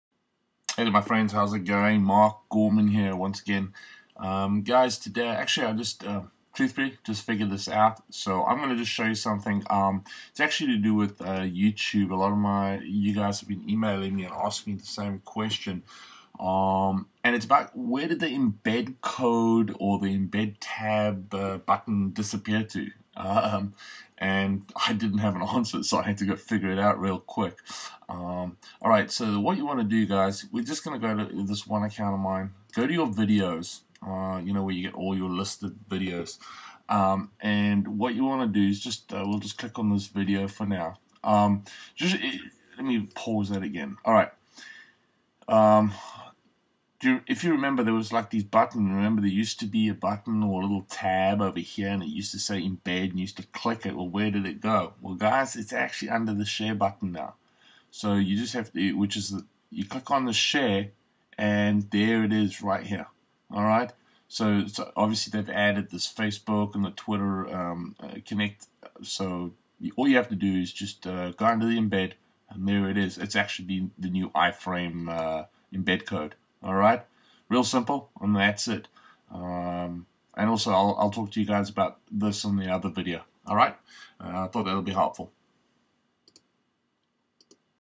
iframe Embed How to find New Code
Are you looking for your iframe embed code? Is it missing. Mark Gorman only found this a hour before shooting this as it had disappeared from it original place. Found under the share button/tab next to the facebook and twitter links. Now you can copy and paste into your wordpress blog.